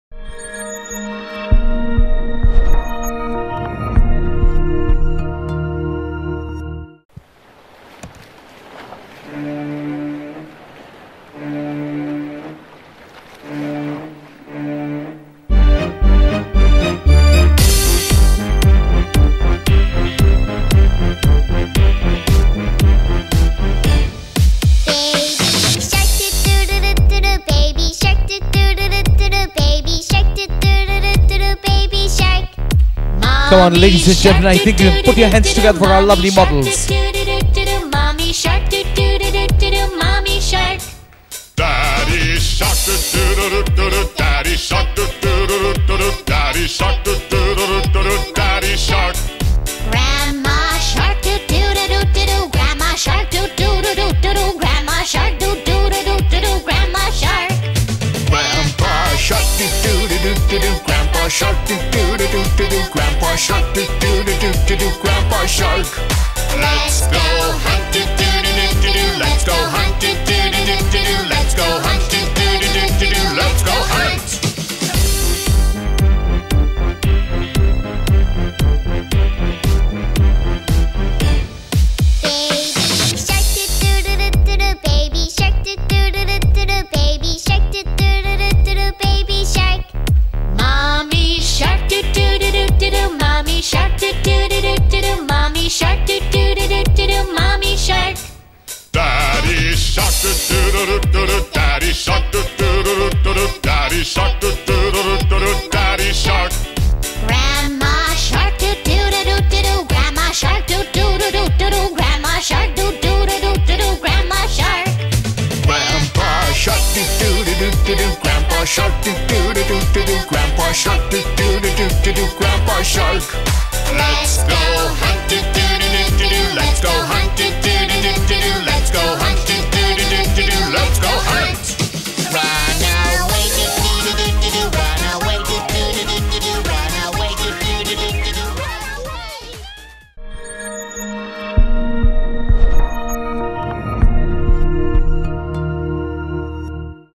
Nemo Returns l Marquee 2019 - Annual Fashion Show l Le Mark School of Art & Design
Inspired by the character of Nemo from the Disney movie Finding Nemo,  this kids wear collection is a reflection of innocence and childhood. Bright colours like yellow and orange are mixed with cool colours like blue, to give a vibrant effect. This cute, Nemo inspired printed resort wear, comprises shorts, skirts  &  capes.  Sustainable cotton fabric makes this collection child friendly.

Designed by our Fashion Designing Students:
Jill P, Neelam M, Neelam Y, Suzan P, Soni B, Hinal M, Reshma M, Neelam B & Krutika Z.